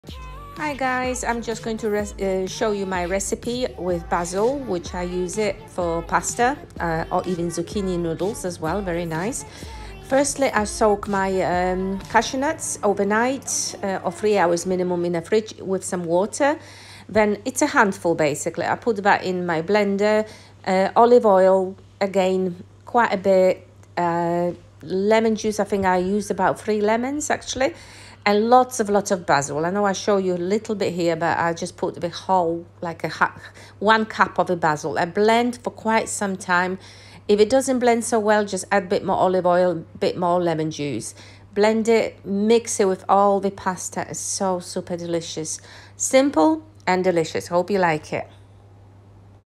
Basil Pesto with out garlic, served with pasta or zucchini noodles
Delicious Basil Pesto Sauce without garlic, perfect with pasta or zucchini noodles eating raw , yummy
Hope you enjoy it as much as me
Benefits of basil; antioxidants and anti-bacterial properties, basil helps maintain skin health, boosts digestion and detoxifies the body. It also helps in the prevention and cure of diseases like cancer, diabetes and other heart conditions. Fresh basil contains Vitamin A, Vitamin K, Iron, Manganese, Calcium, and essential oils.